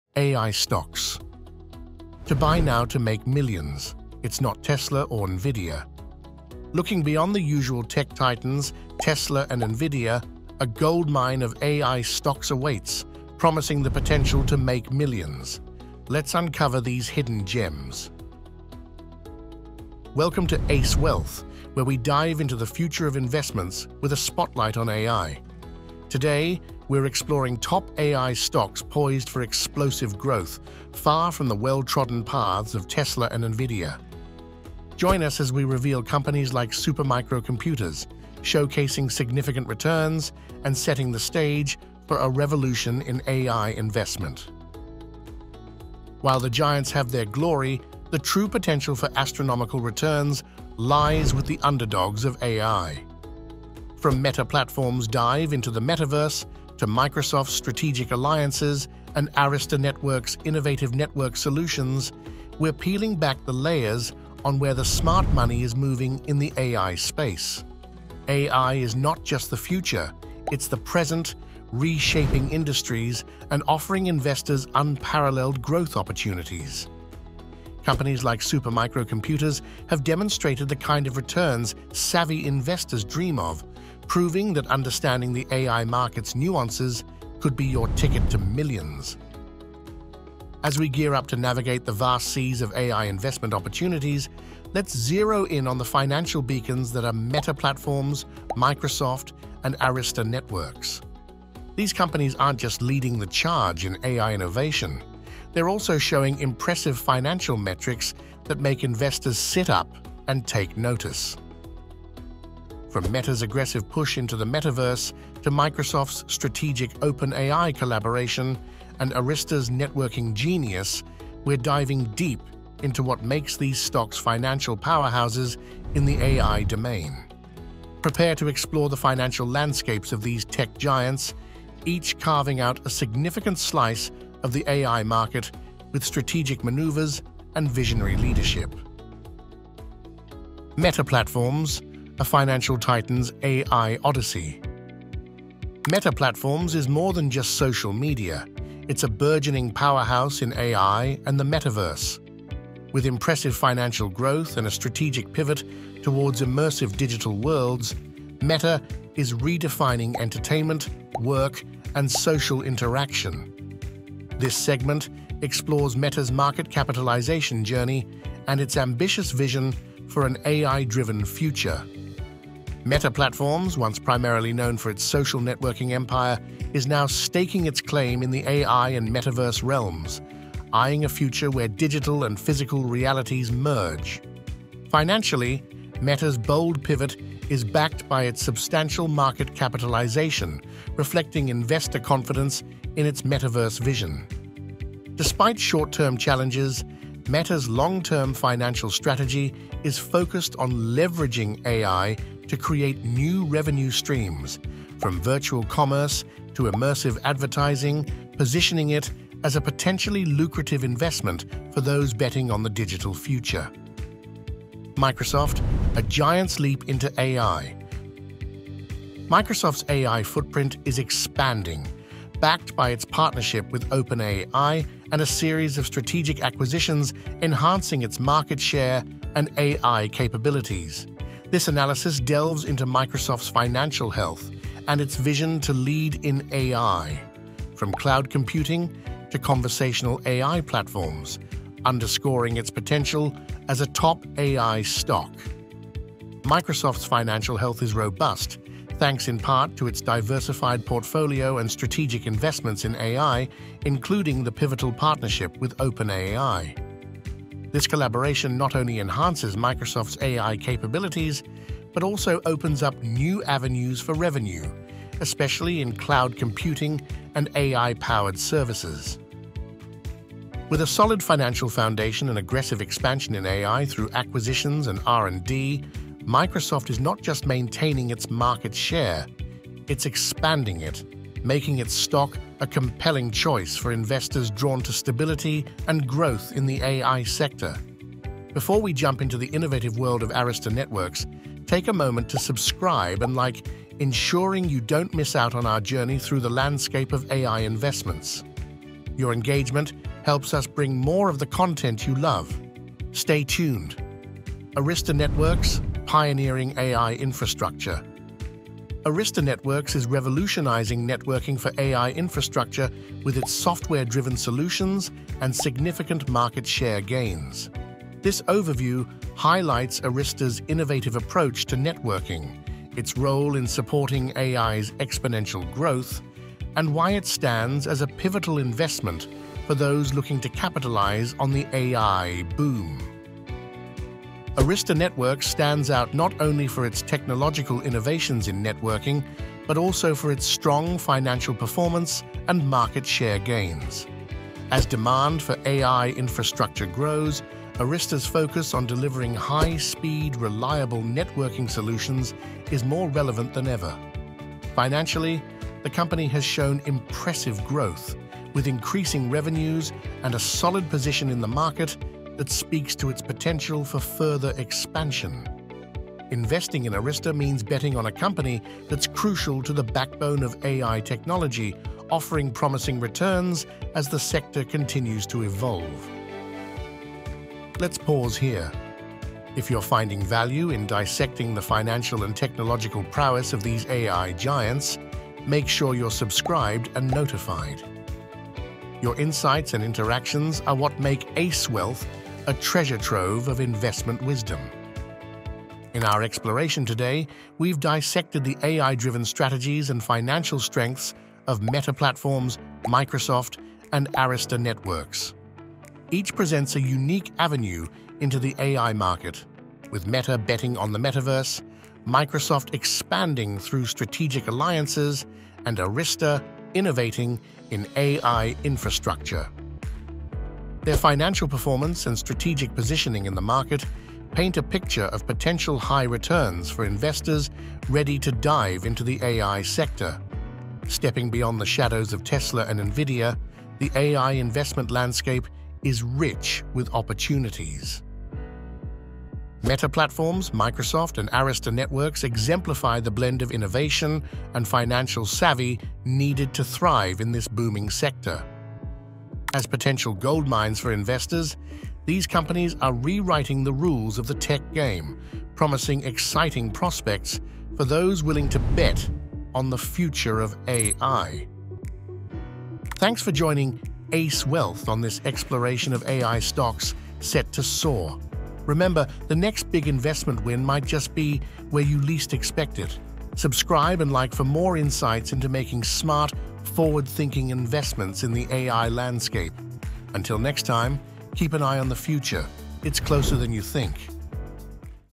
AI Stocks To Buy Now $$ 🤯 - It's Not Tesla Or NVIDIA Stock
💰 Welcome to ACE Wealth Strategies, your premier destination for mastering the art of stock investing! In our latest video, "Best AI Stocks To Buy Now To Make Millions 🤯 - ( It's Not TSLA OR NVDA)," we unveil the untapped potential of AI stocks poised for exponential growth. Explore innovative AI companies beyond the usual suspects like Tesla and Nvidia, and uncover the hidden gems that could lead to massive wealth accumulation. Join us as we delve into the transformative power of artificial intelligence in shaping the future of investing. 📈💼